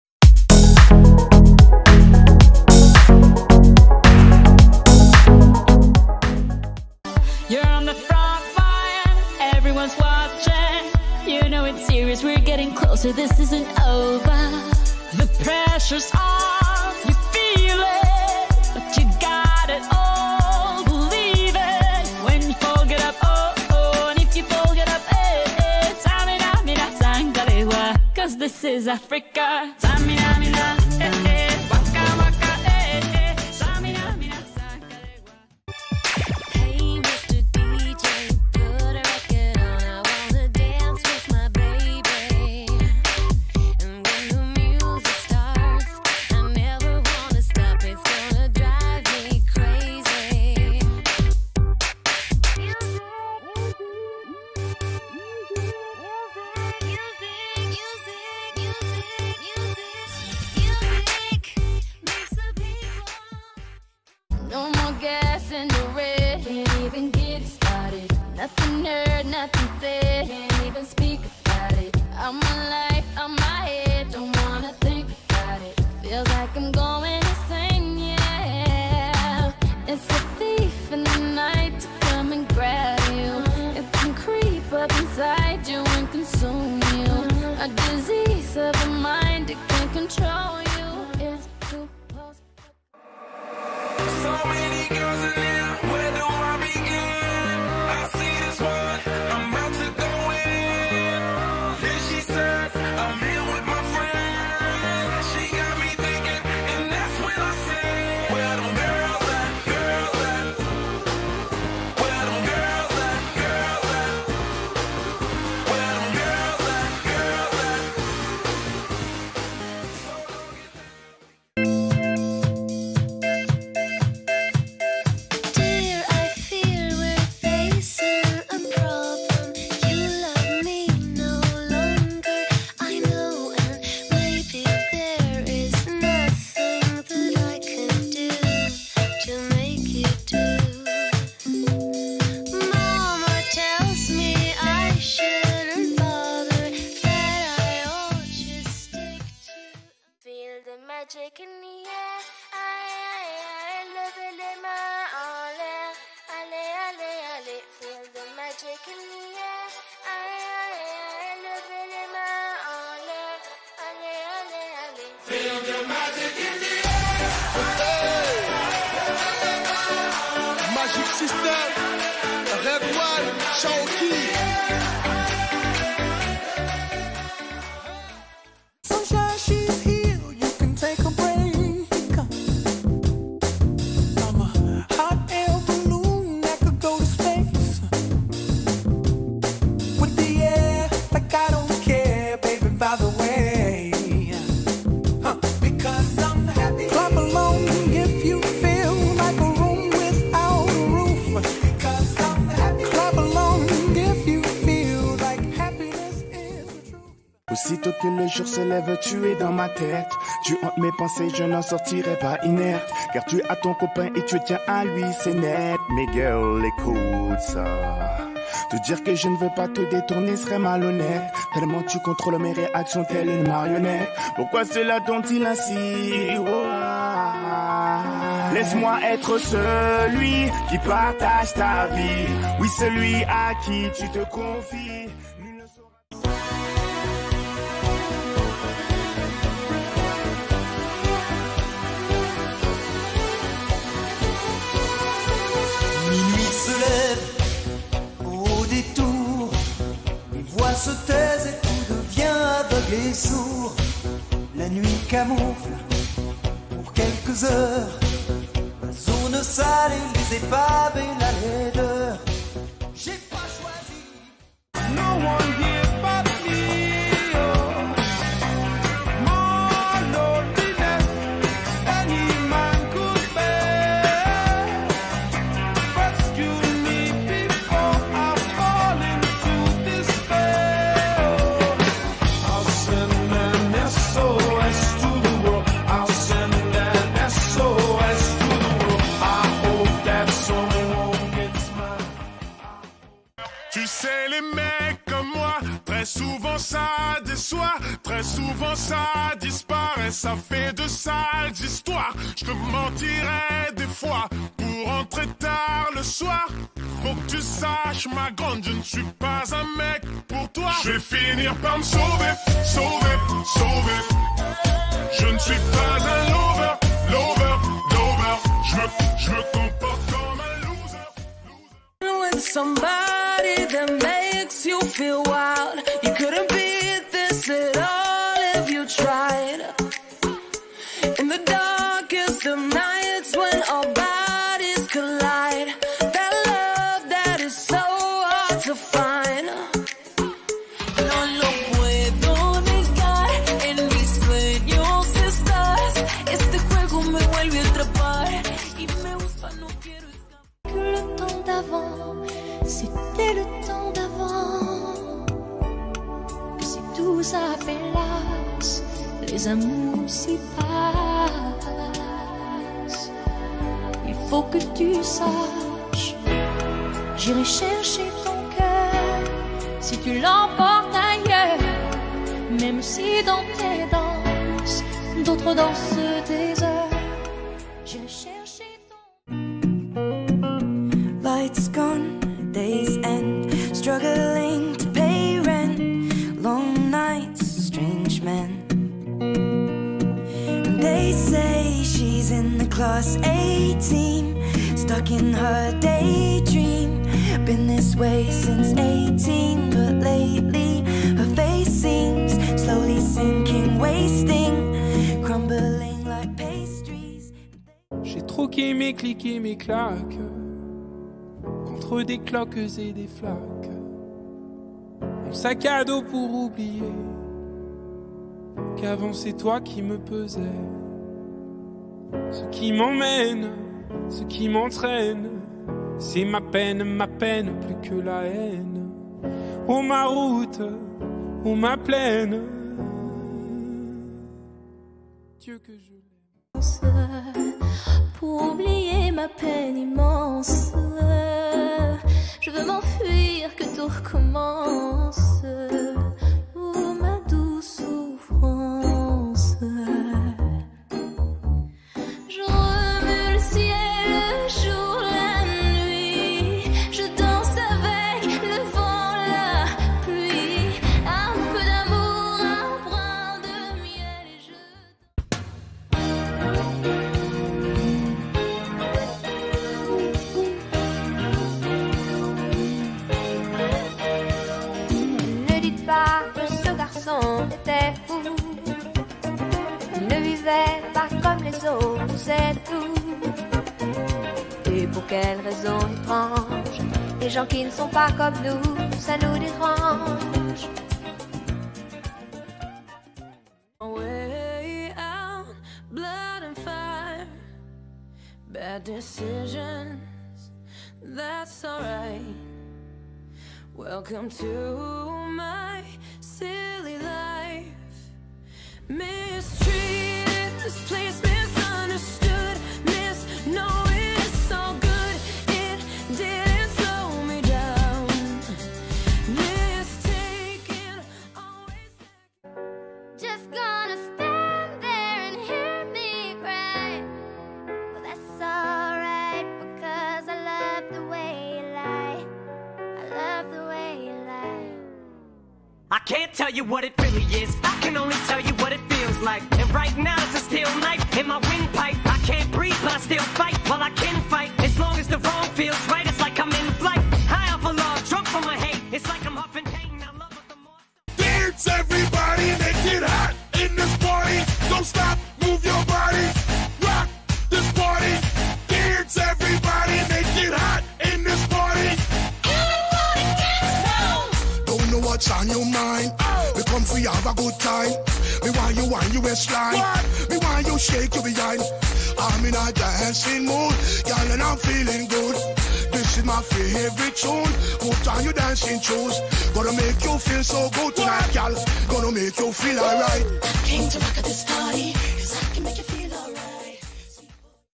Blind Test #112 Toutes générations Années 80 à aujourd'hui
- Blind Test Toutes générations Années 80 à aujourd'hui
- 20 extraits et 16 secondes pour trouver le titre et l'artiste

N'OUBLIEZ PAS
De laisser un 👍 bleu si vous avez aimé !
Merci à ceux qui le font !

Vous pouvez aussi laisser vos scores 🏆 et vos idées en commentaires 💬.